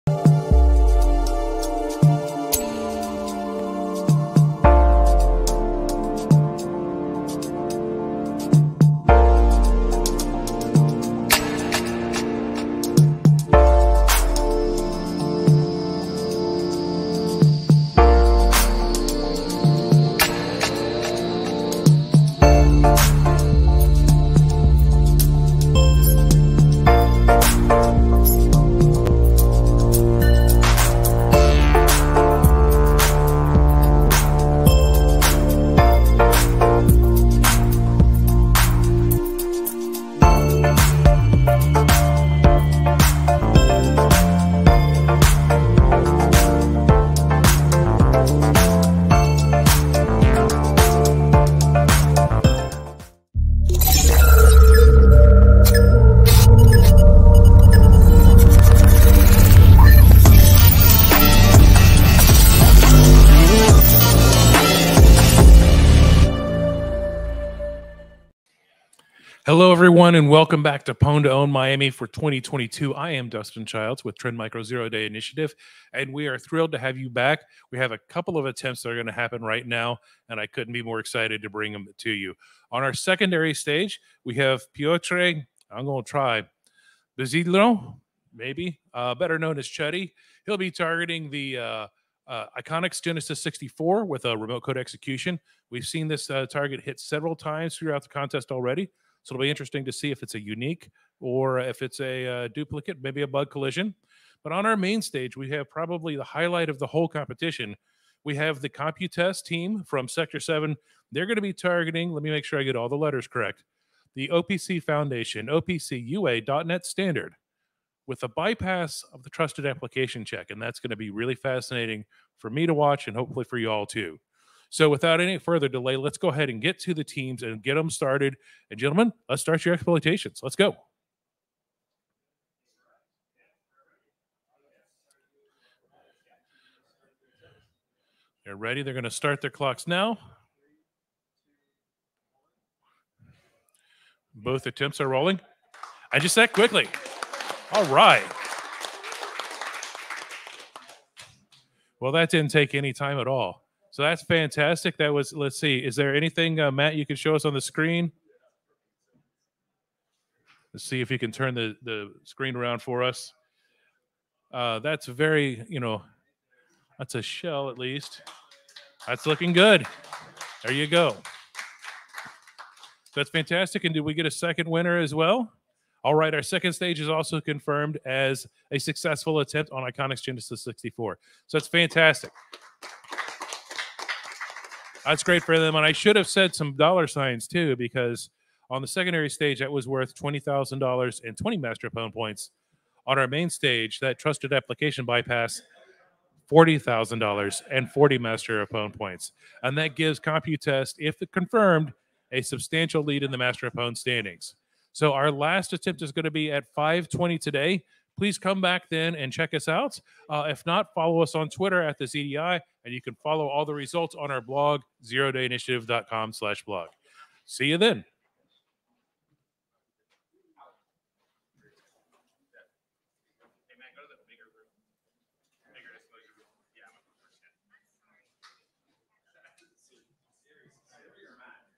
Pwn2Own Miami 2022 - Computest Sector 7 vs the OPC Foundation OPC UA .NET Standard
Our first streaming attempt of Day 2 of Pwn2Own Miami 2022 will have Daan Keuper (@daankeuper) and Thijs Alkemade (@xnyhps) from Computest Sector 7 (@sector7_nl) targeting the OPC Foundation OPC UA .NET Standard with a Bypass of the Trusted Application Check in the OPC UA Server category. If successful, they'll earn $40,000 and 40 Master of Pwn points.